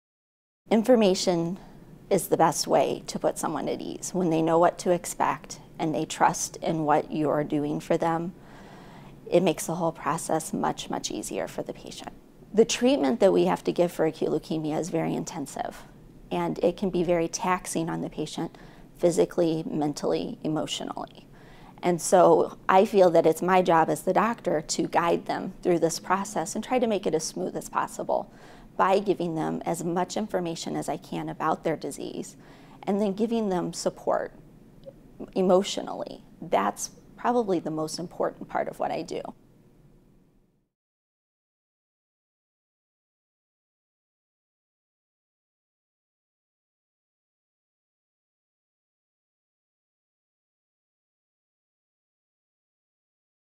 Melissa Larson, MD, Hematologist/Oncologist at RUSH
Melissa Larson, MD, is a hematologist/oncologist with RUSH. She specializes in the treatment of leukemia. In this video, Dr. Larson discusses providing patients with clear information, which helps alleviate their anxiety and builds trust in the medical process. In treating acute leukemia, she emphasizes comprehensive guidance and emotional support, aiming to ease the physical, mental, and emotional toll of intensive treatments.